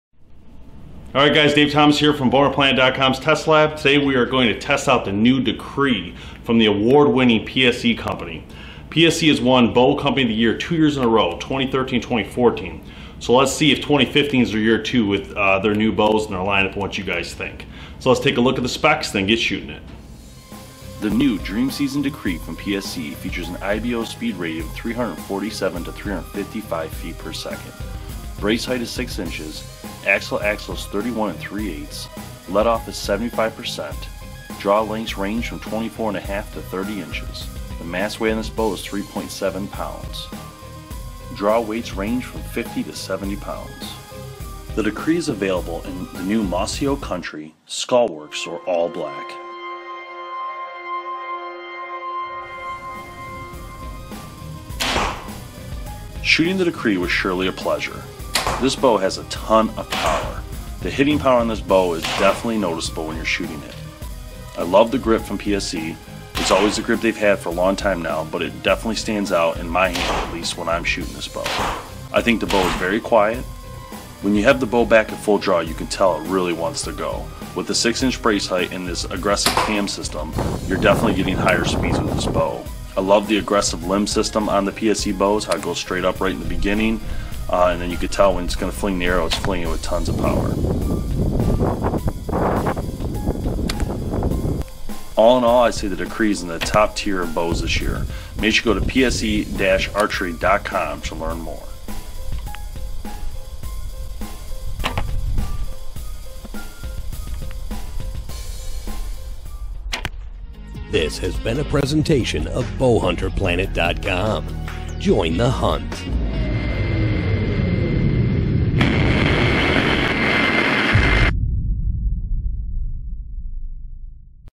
2015 PSE Dream Season Decree
Check out the all new 2015 PSE DreamSeason Decree. A fast shooting bow that packs a huge punch.